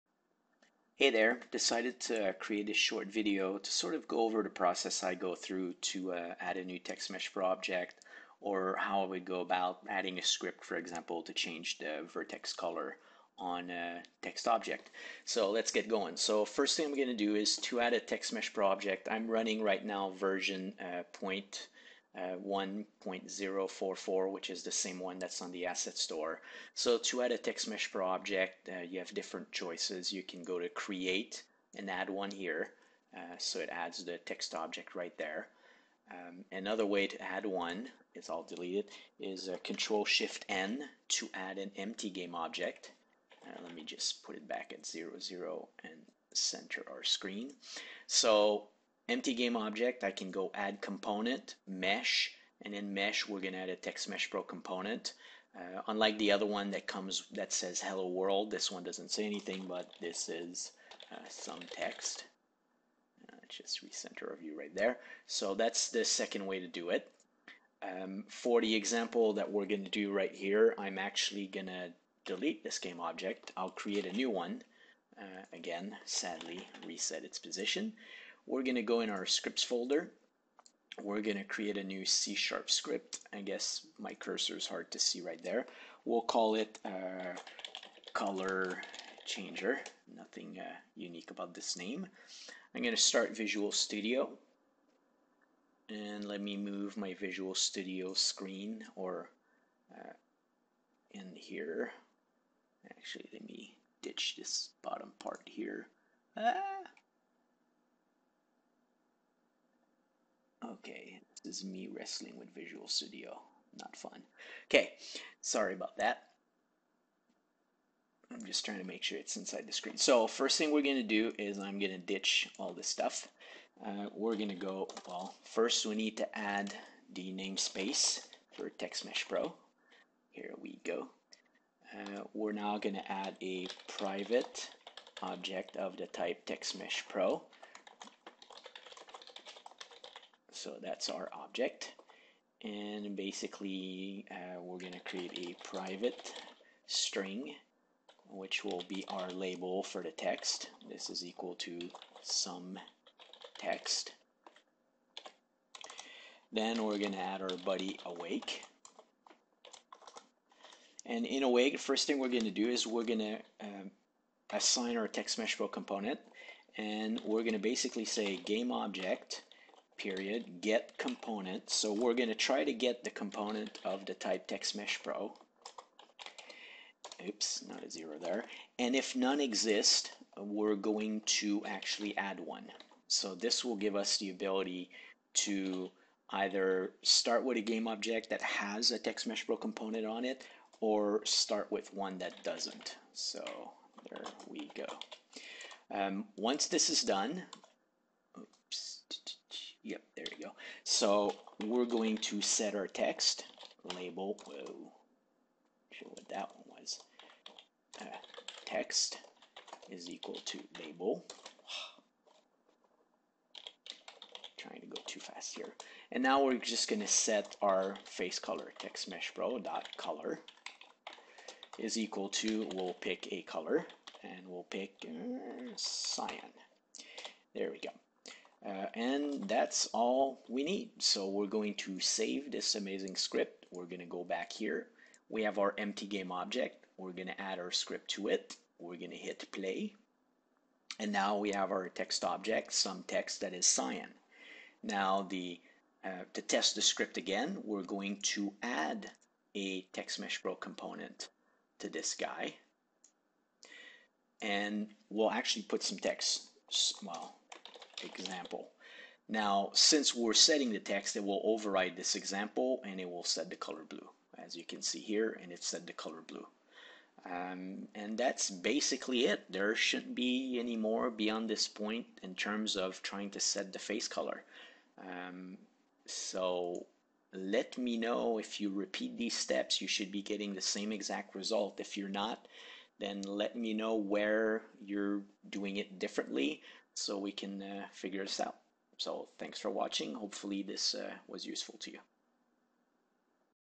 TextMesh Pro - Changing Vertex Color
Simple example of how to add a new TextMesh Pro object into a scene as well as an example of how to add a new object via scripting and changing the vertex color of the object.